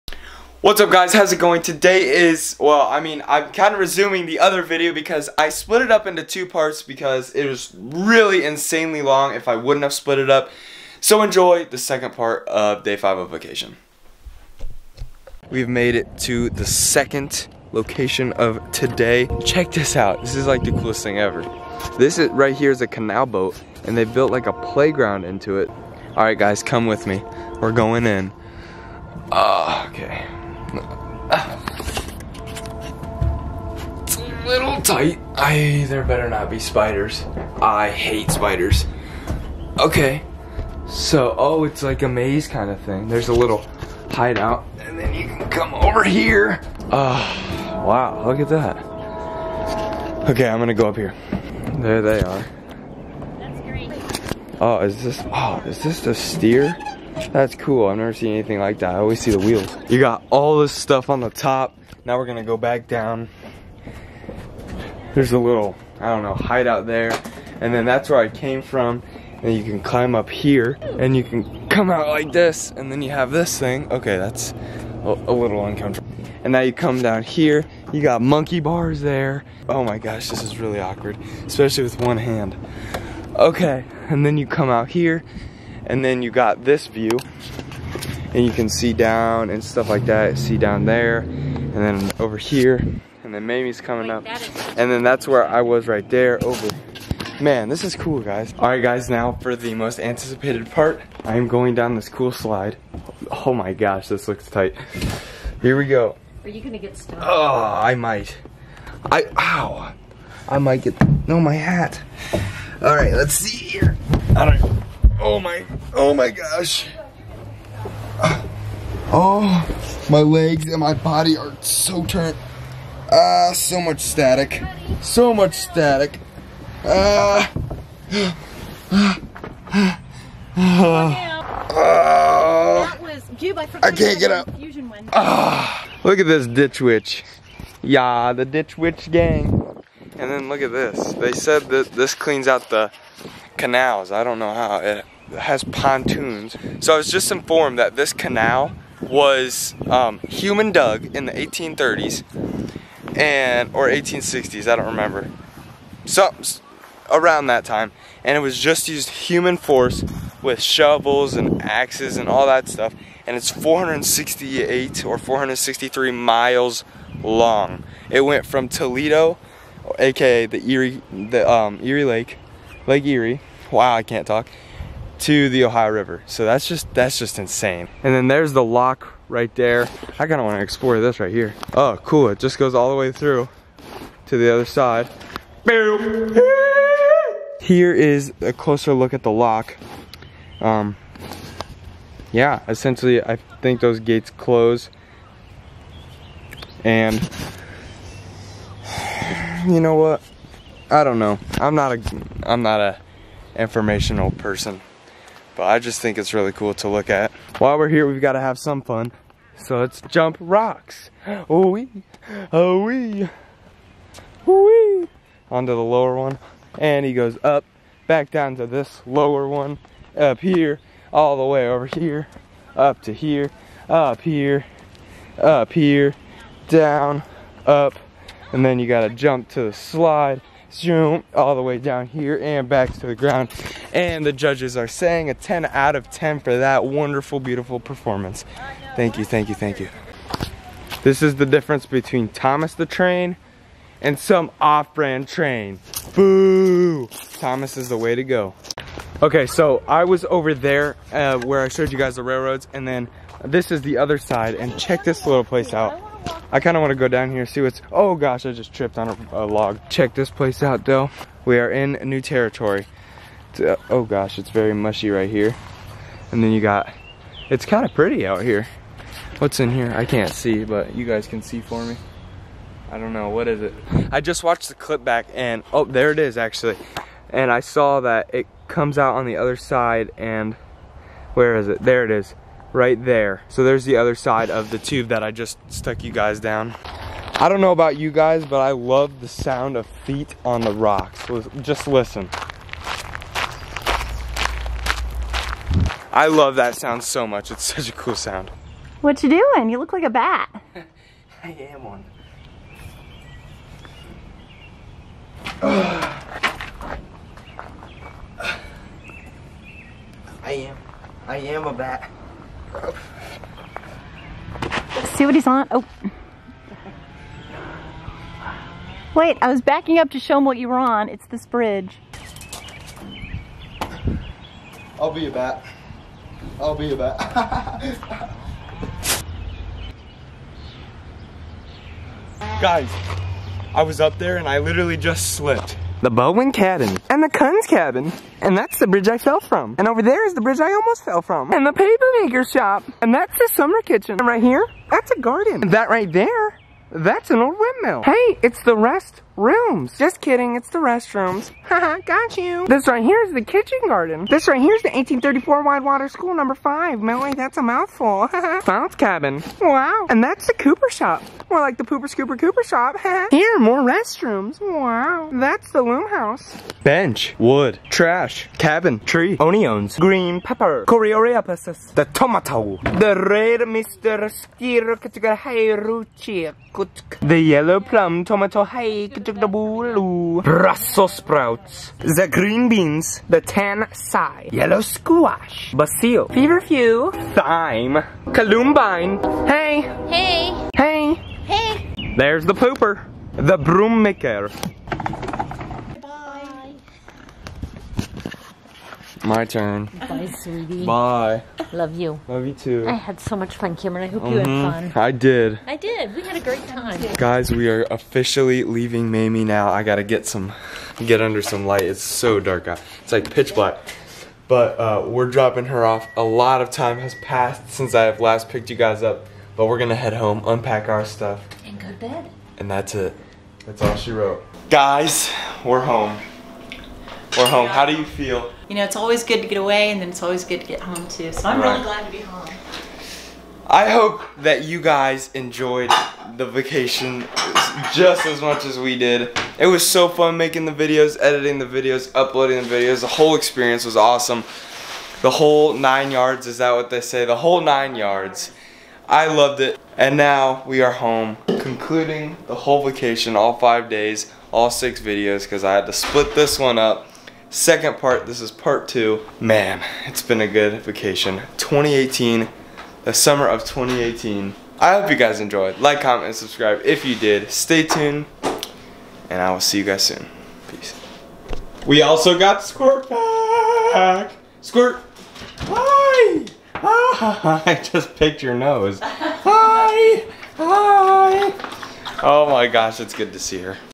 I ALMOST FELL OFF OF A BRIDGE!!
Today was the last day of vacation and also part 2 of the video. It was really fun except for the fact that I slipped and almost fell off of a bridge. I hope you enjoyed!